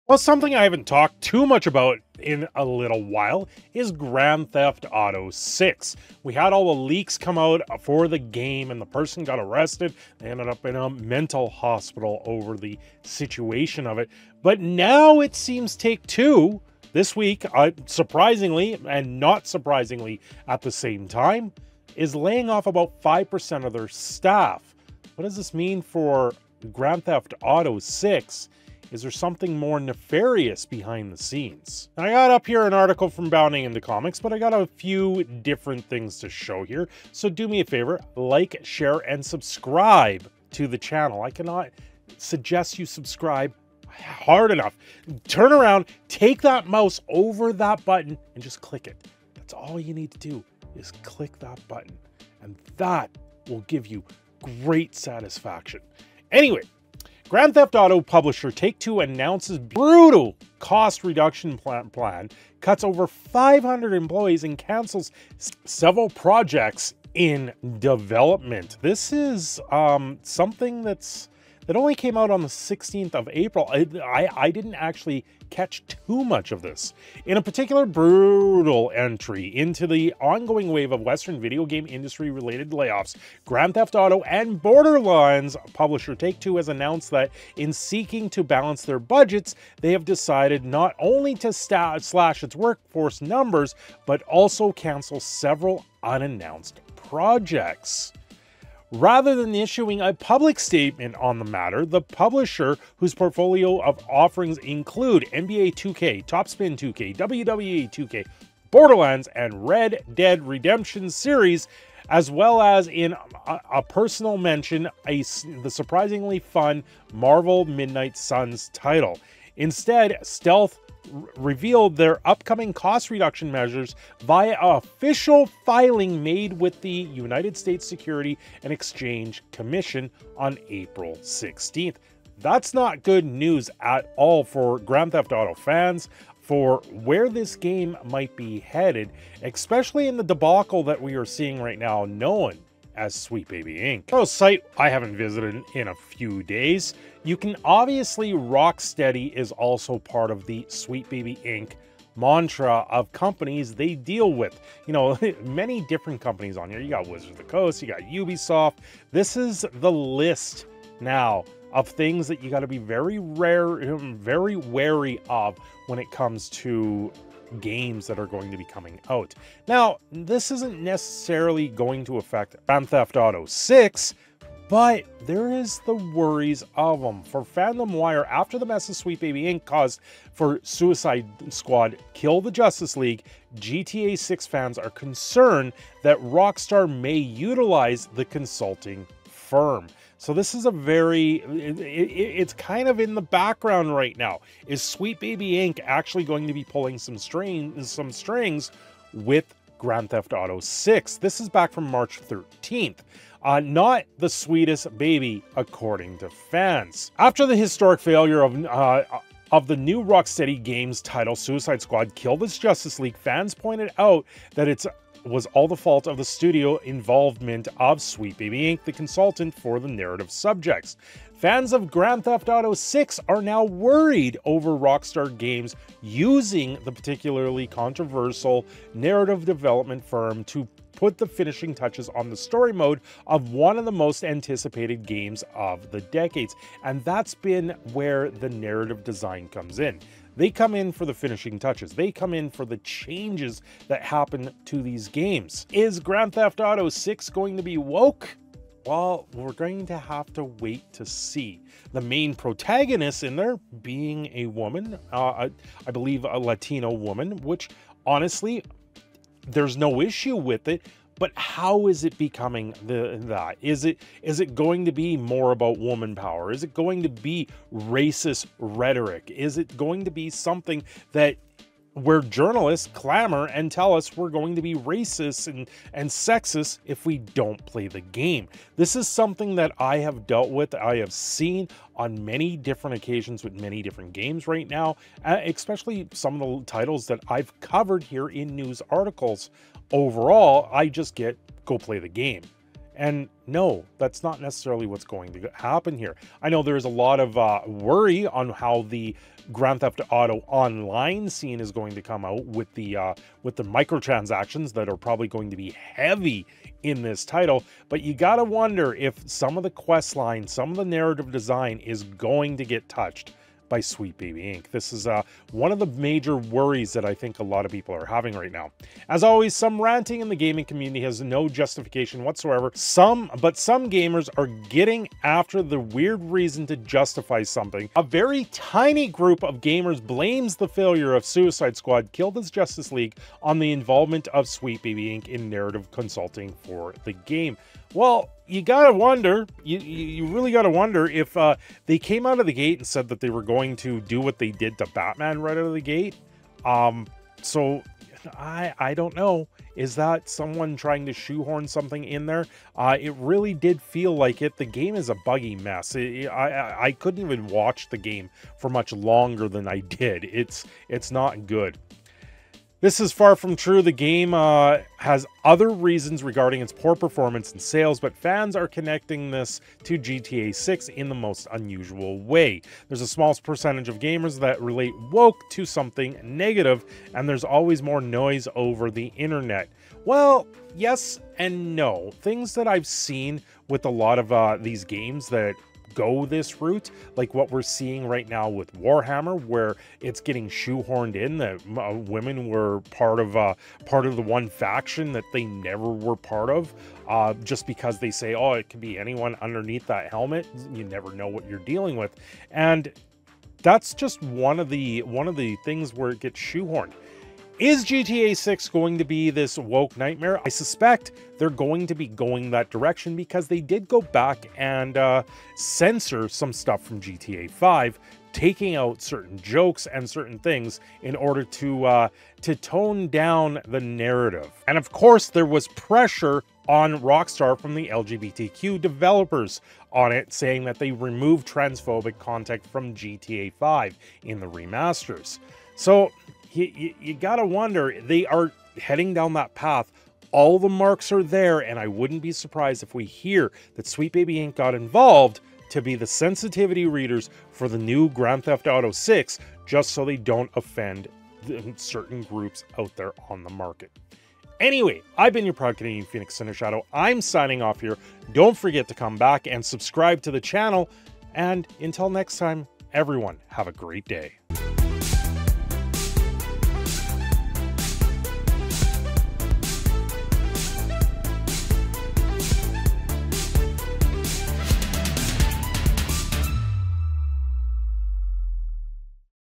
With New Layoffs For Take-Two Will Narrative Design Take Over For The Making Of GTA 6
lots of worry that Sweet baby Inc is involved with Rocksteady over Grand Thief Auto 6.